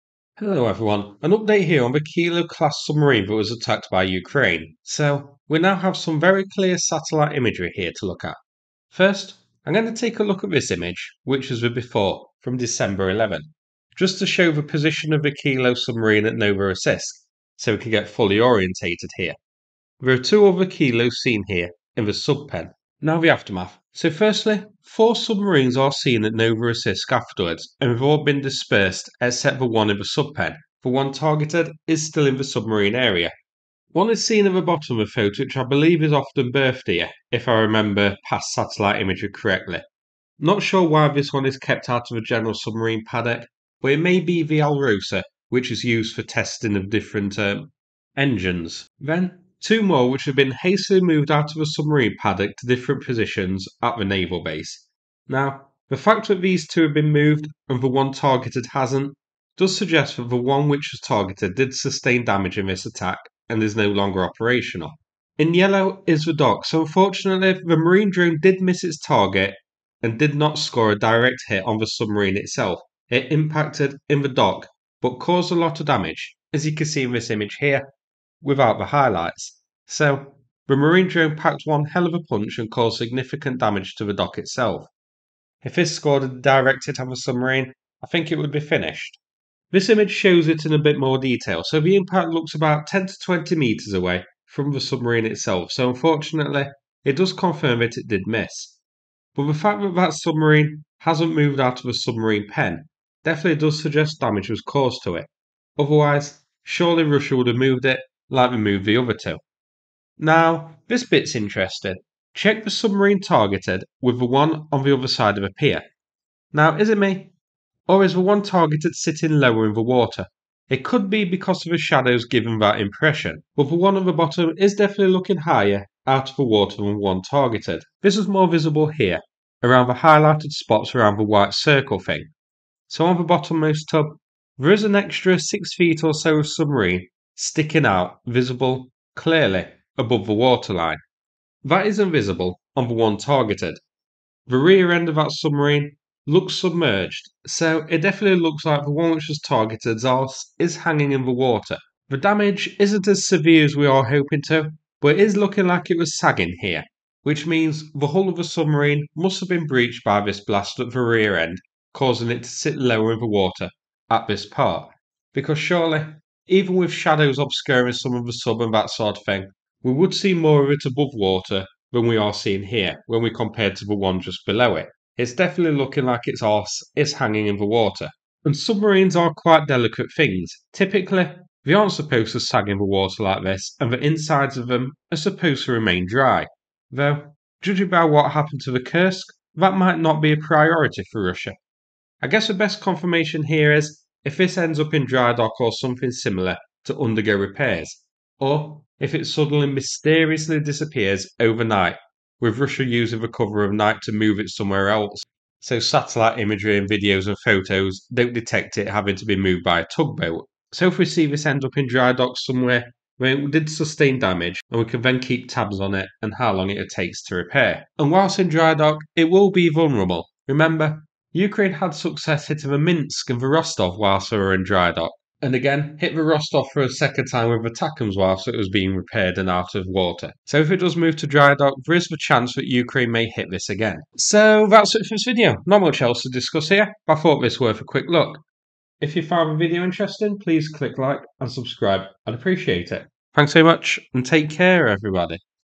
Satellite Imagery of Kilo-Class Submarine at Novorossiysk--Possibly Sagging at the Rear
Satellite imagery of the Kilo-class submarine attacked by Ukraine potentially shows that the rear is sitting lower in the water compared to the one nearby. It could be shadows, but it is hard to tell for certain.